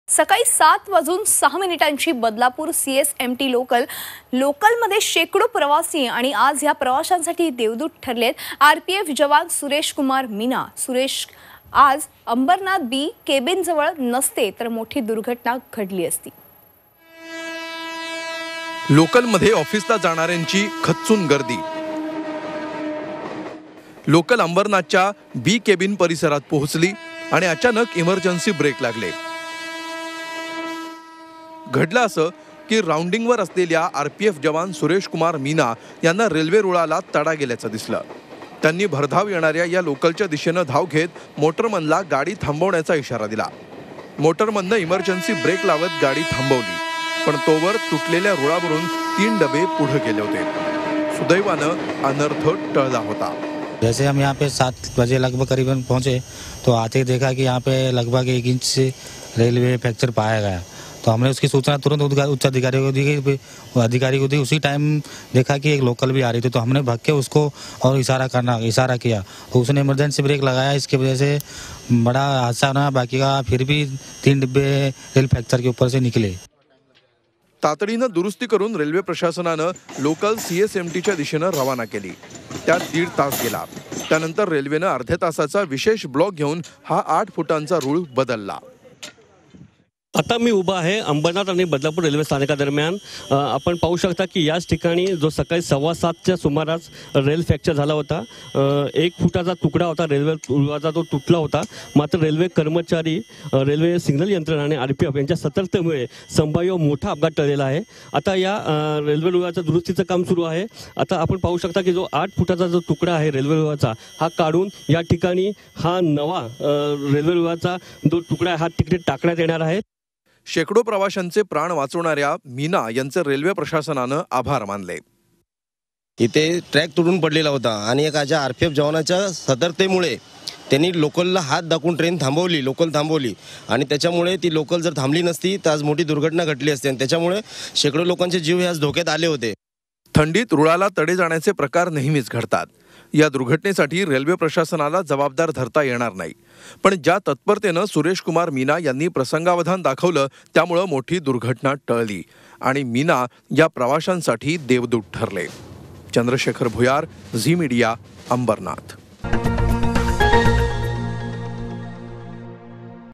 सुरेशकुमार मीना ठरले शेकडो प्रवाशांसाठी ठरले देवदूत
Ambernath | Central Railway | Big Mishape Was Save By RPF Jawan Sureshkumar Meena